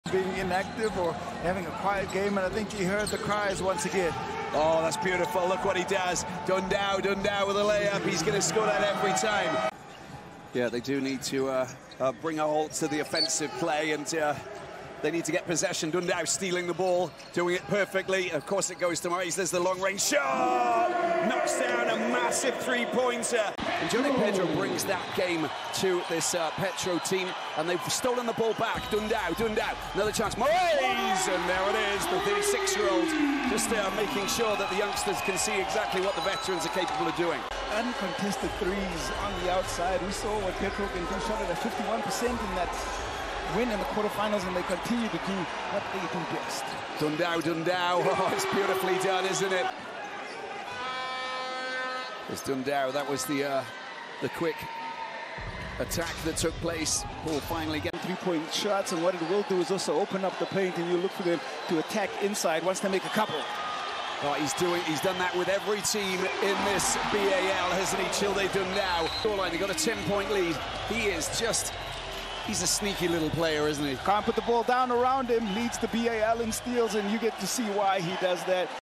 BAL Season 2 Top Steals - CHILDE DUNDAO
Childe Dundao is one of a kind these days. Game after game, he went from strength to strength in Petro’s rotation. Ending his second participation in the BAL with an average of 3 steals per game! With his speed and precision, Dundao was named to the BAL All-Defensive Team while helping his Petro de Luanda to second place in the tournament.